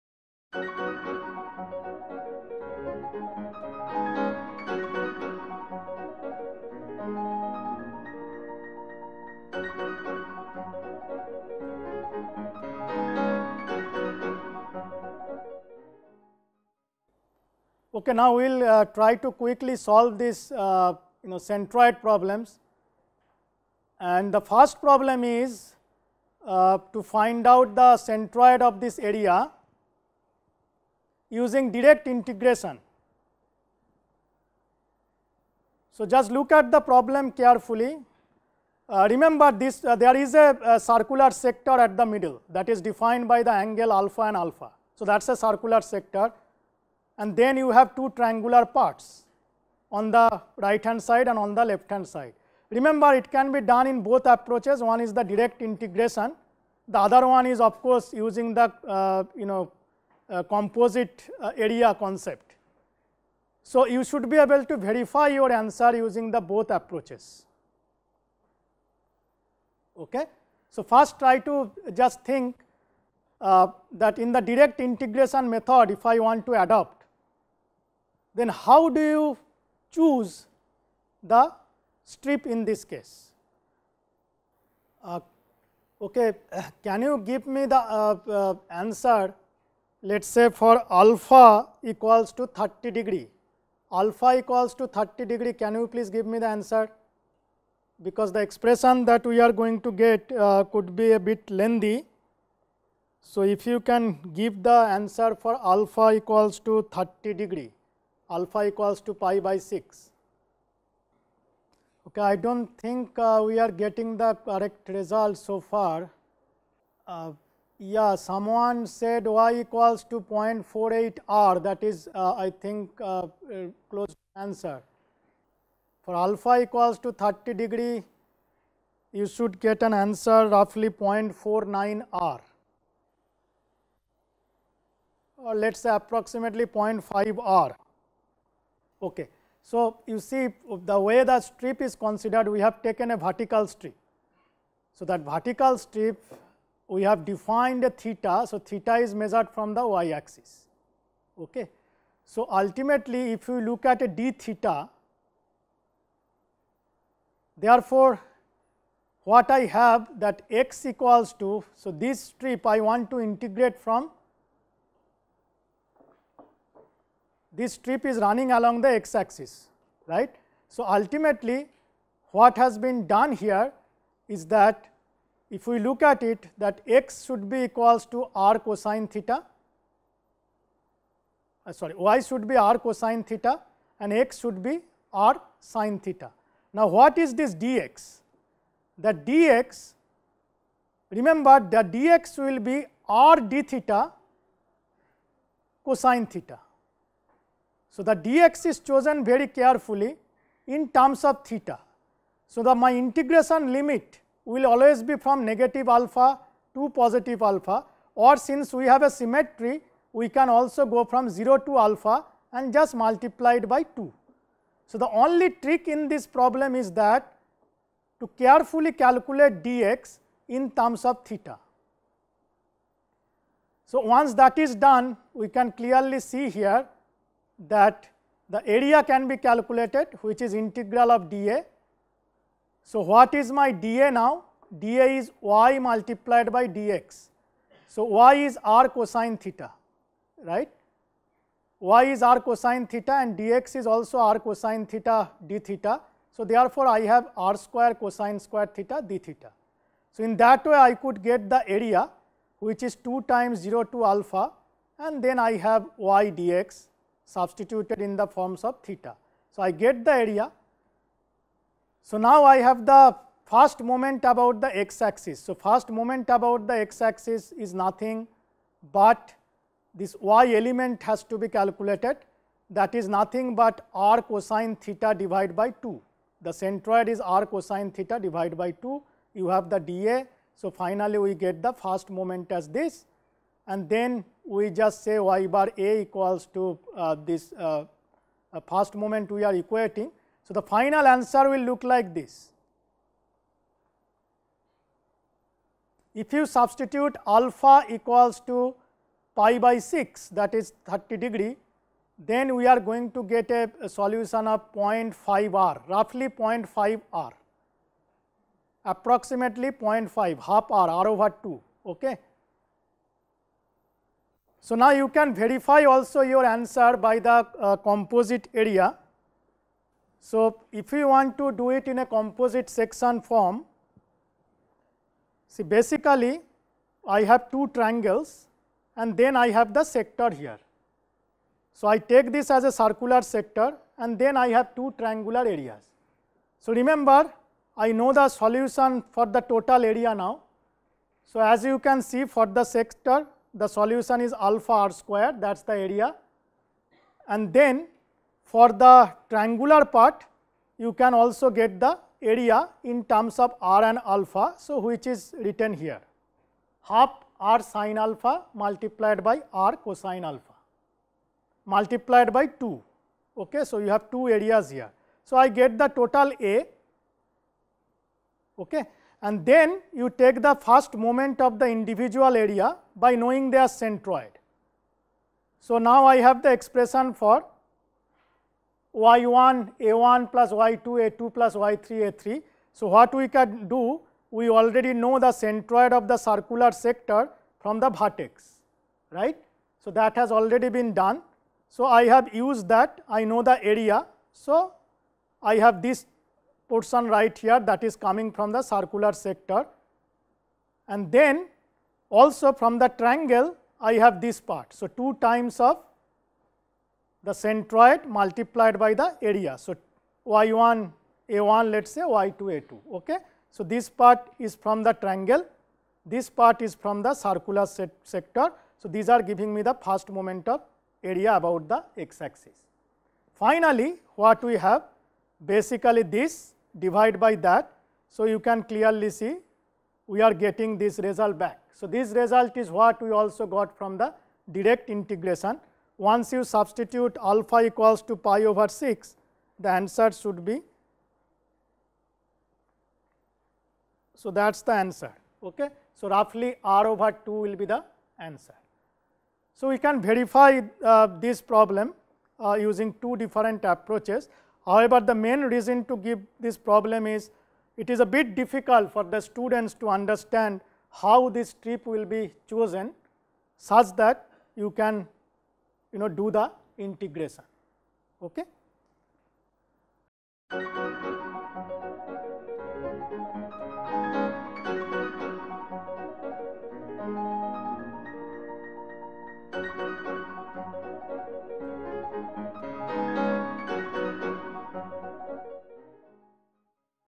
EM M L4B Centroids and Centre of Gravity ( Tutorial )
This is part B of 4th session of Engineering Mechanics workshop arranged for teachers. It was delivered by Prof. Sauvik Banerjee from IIT Bombay.

In this session he solved different tutorial problems on Centroids and Centre of Gravity and afterwards explained the same in details

The syntax used in the video title is as follows :
EM - Engineering Mechanics
M - Main Workshop
L4B - Lecture 4B

This workshop had been arranged for teachers at various Remote Centers. A large number of participating teachers were assembled at nearest Remote Centers of their colleges where IIT Bombay faculty delivered live lectures from IIT Bombay in distance mode through A-VIEW software.